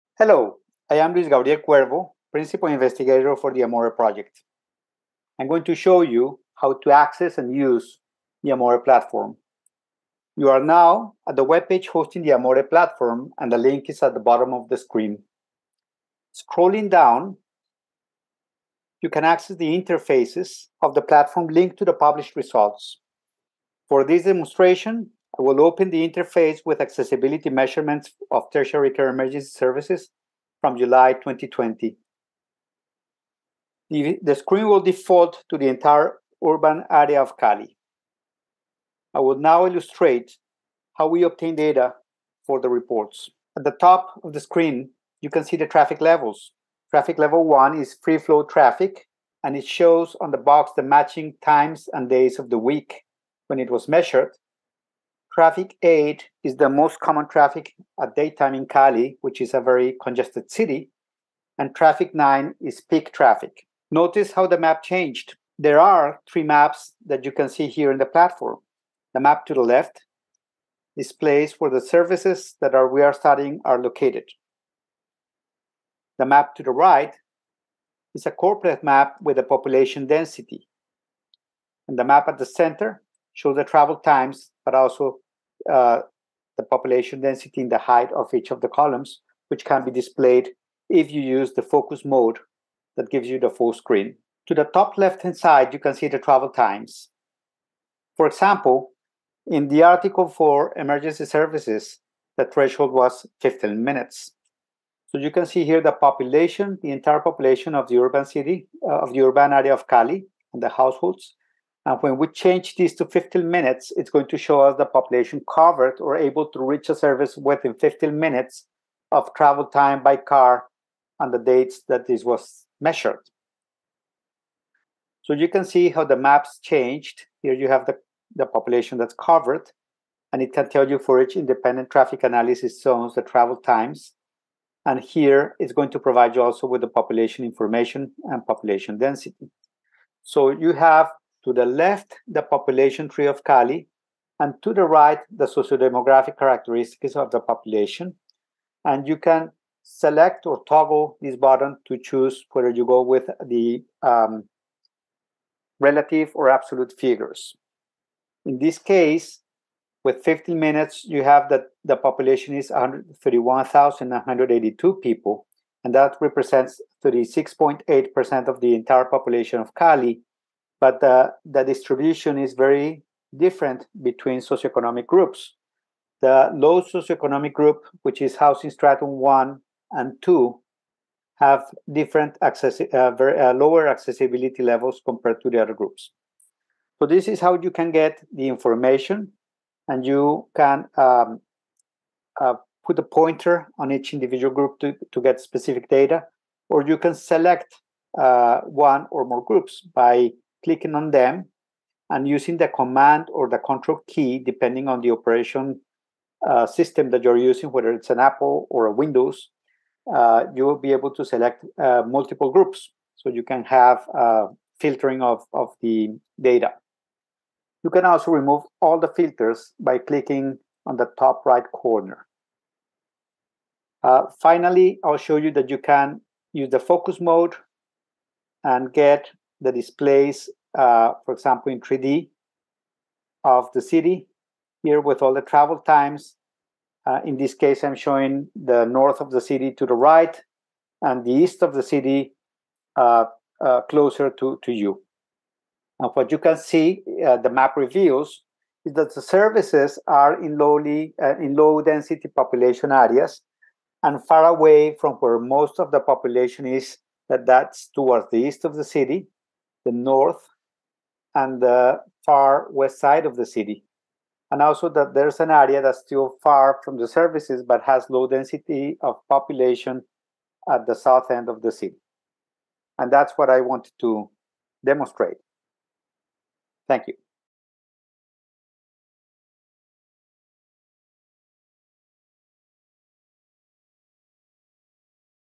Demo AMORE Platform
To access and use the AMORE Platform interfaces for published results The website interface is in English or Español.